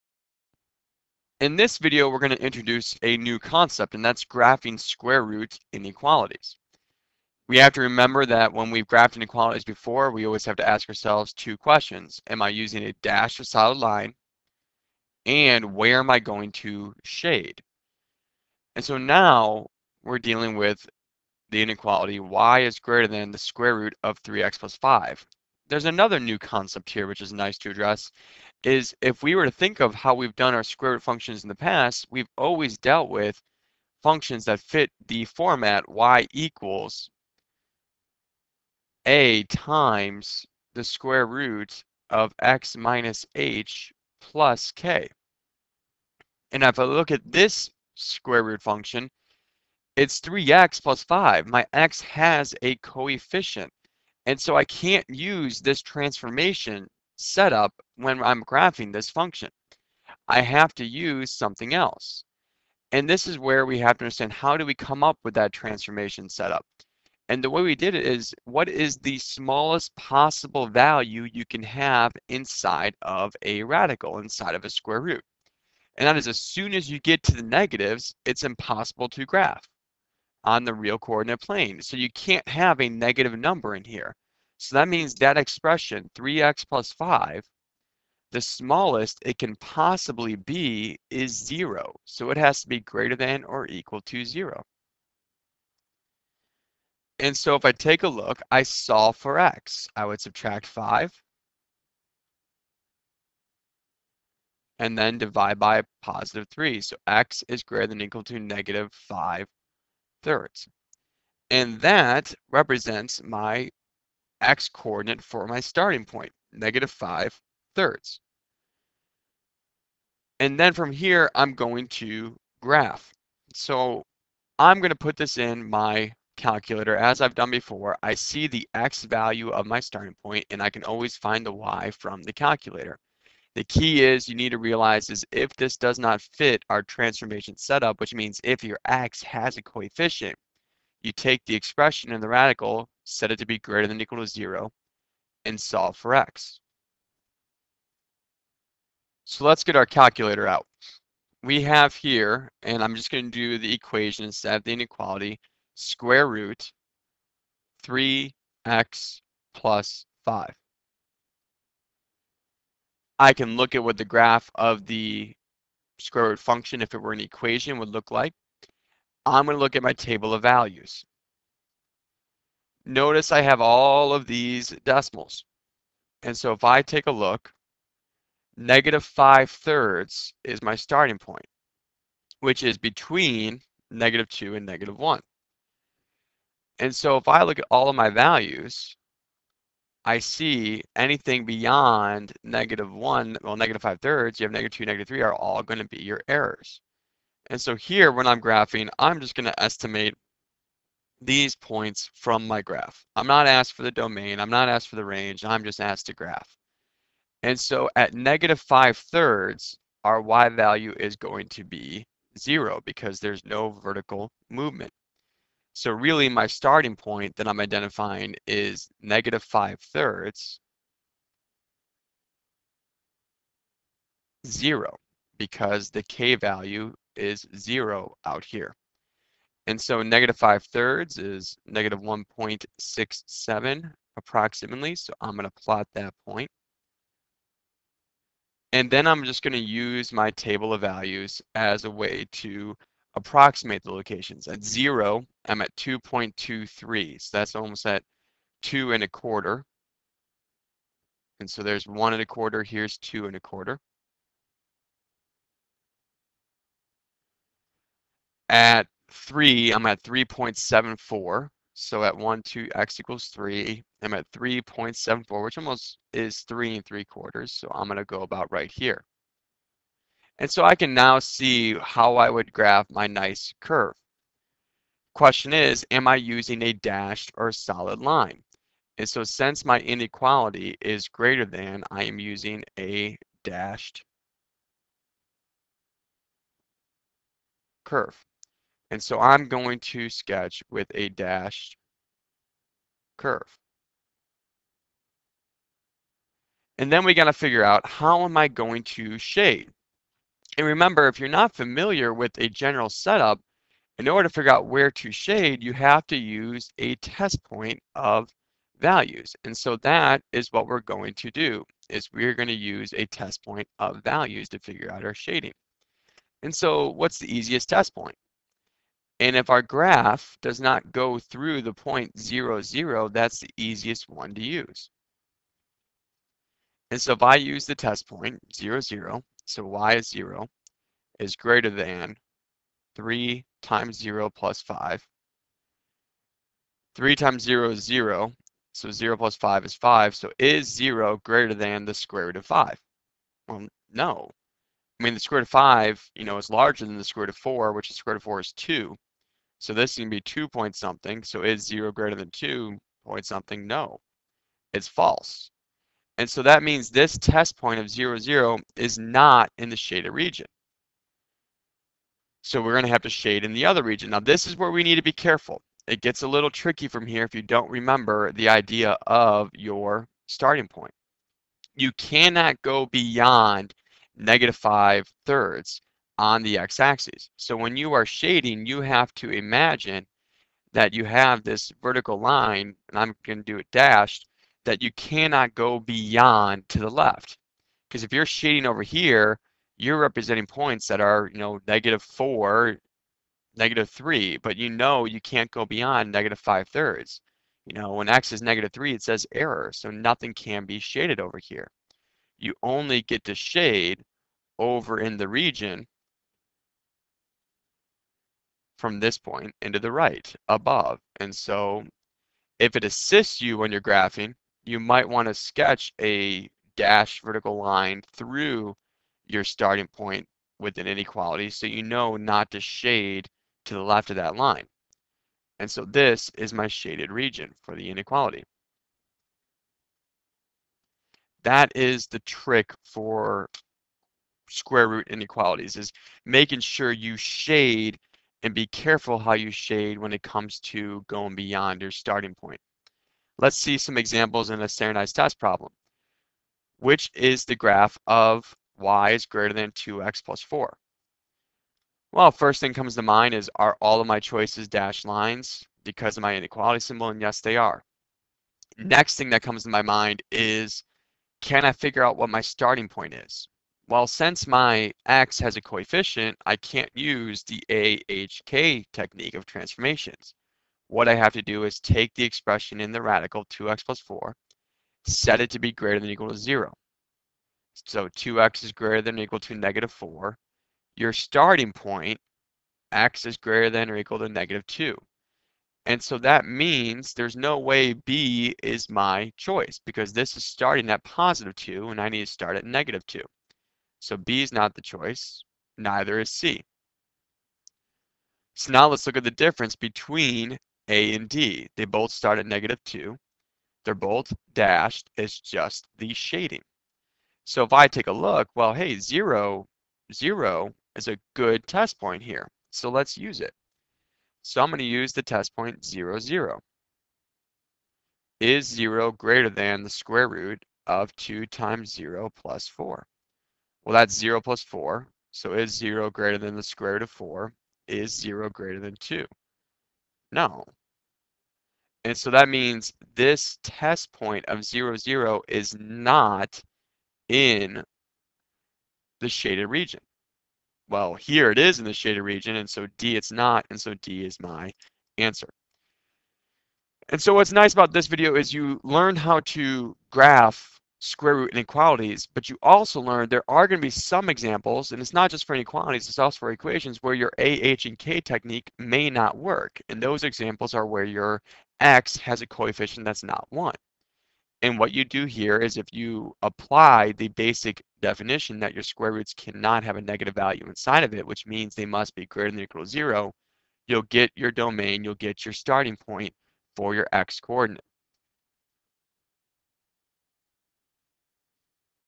Lesson 7.3 - Square Root Inequalities
The following video goes over how to graph square root inequalities.